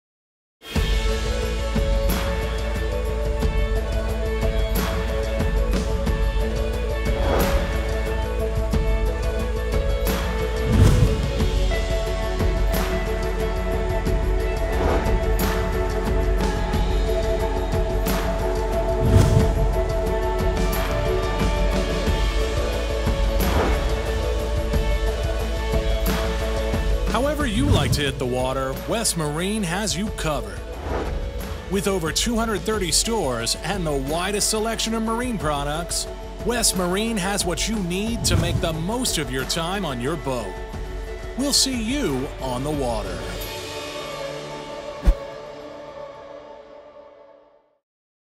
Get Out on the Water with West Marine
However you like to hit the water, West Marine has you covered. With over 230 stores and the widest selection of marine products, West Marine has what you need to make the most of your time on your boat.